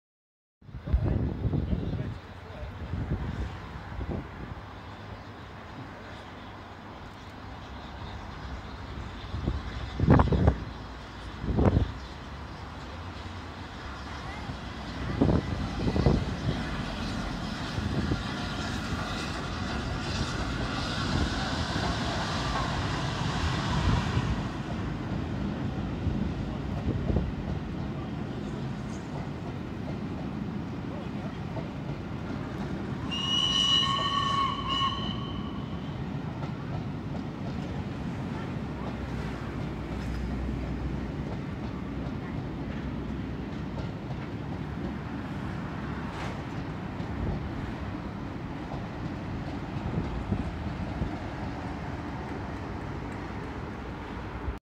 Flying Scotsman at Doncaster
Flying Scotsman with a Scarborough to kings cross charter with the sunny shine out